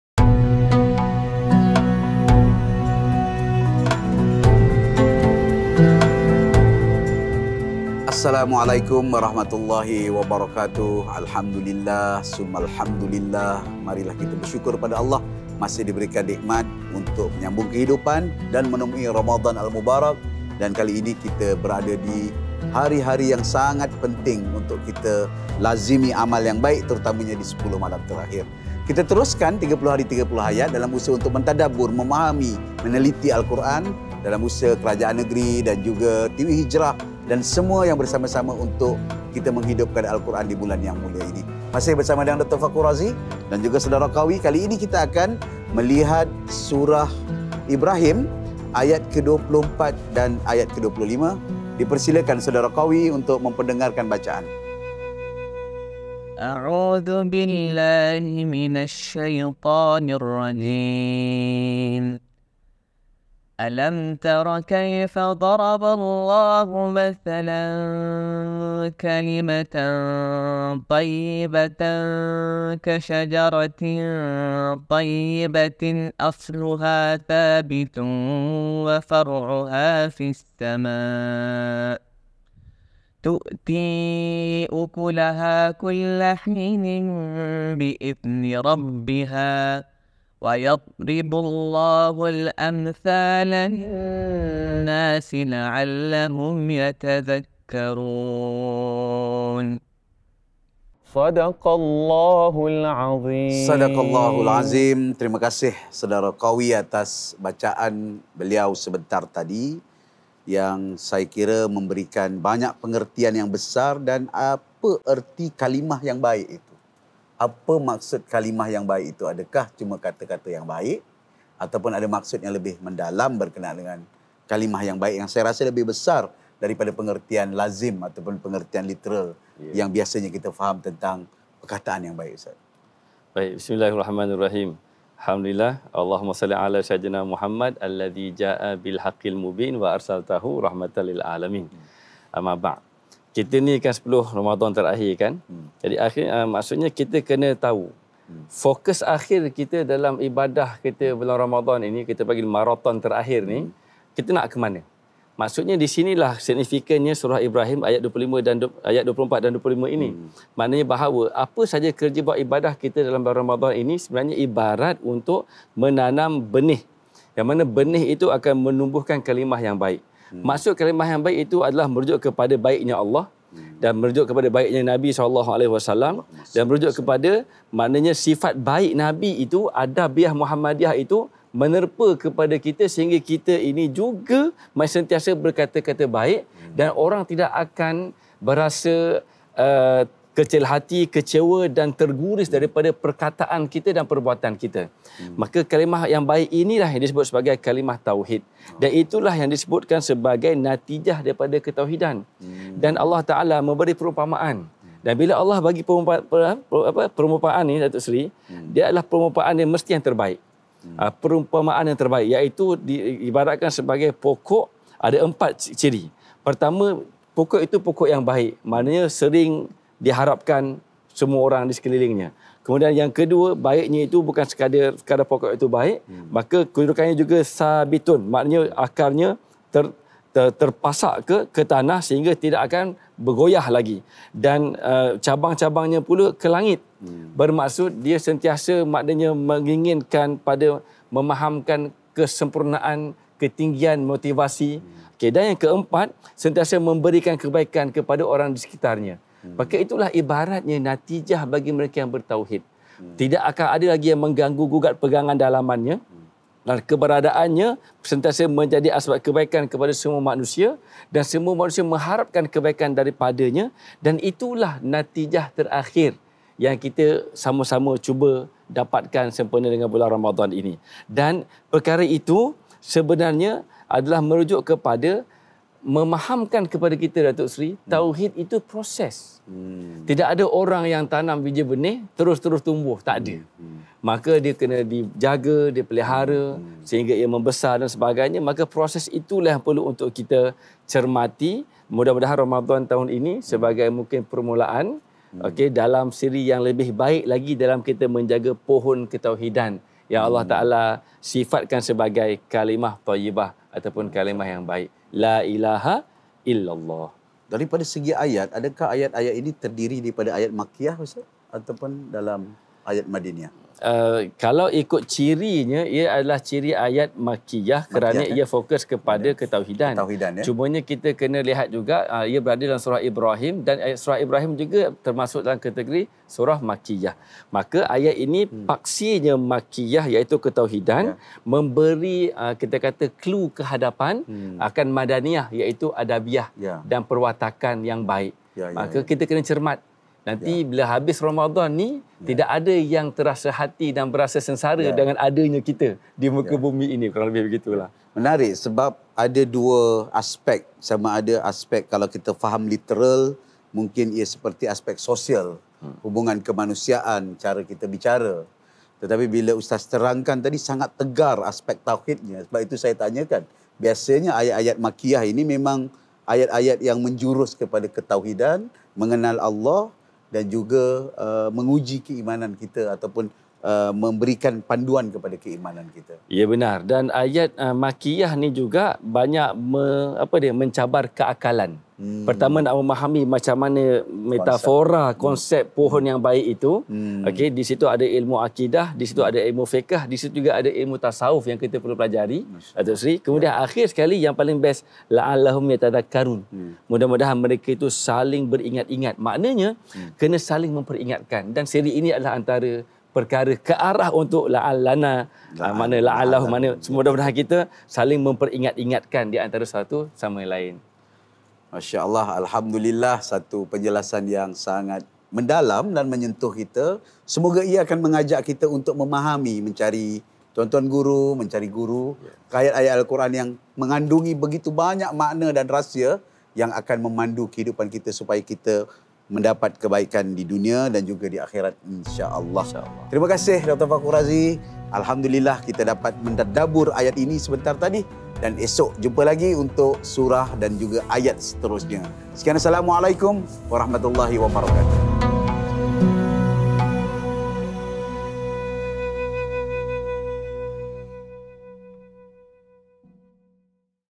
Episod 23 Surah Ibrahim Ayat 24 25
PERUMPAMAAN POKOK DALAM KEHIDUPAN

Hari ini kita meneliti surah Ibrahim ayat 24-25 yang menjentik akal kita tentang perumpamaan Allah SWT tentang tauhid, iman dan amal kebaikan.